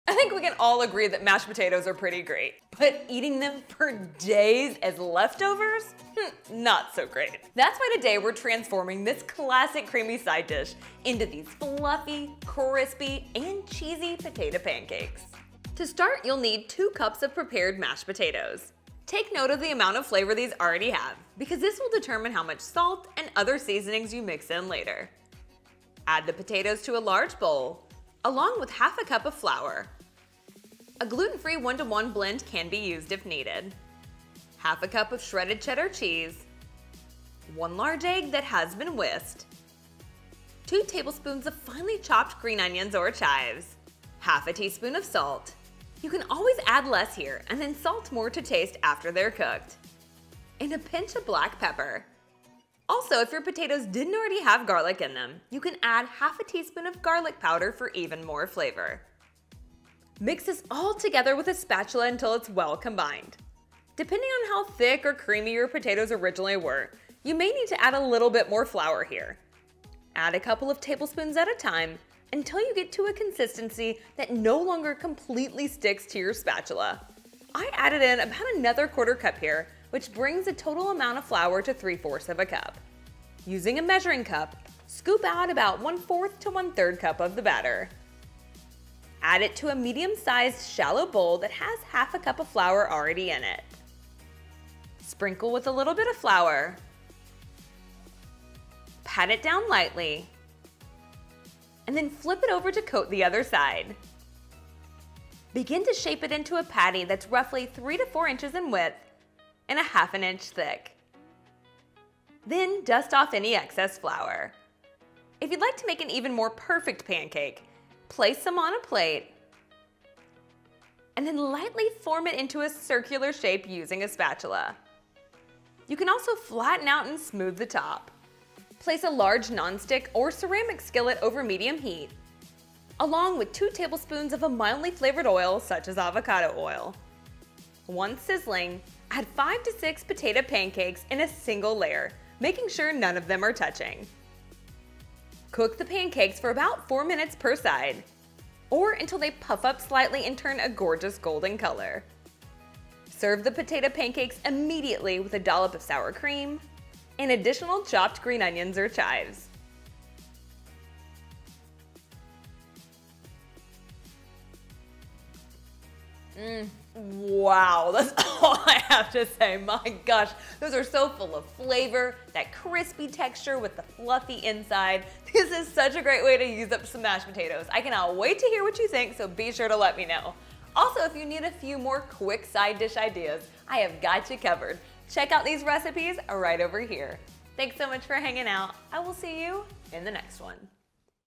Leftover Mashed Potato Pancakes (Fluffy & Crispy!)
Mashed Potato Pancakes are the perfect way to use up Thanksgiving leftovers! It is surprisingly easy to mix mashed potatoes with flour, egg, cheese, and seasonings and cook them up in a skillet. This recipe results in fluffy yet crispy pancakes perfect for a lazy morning.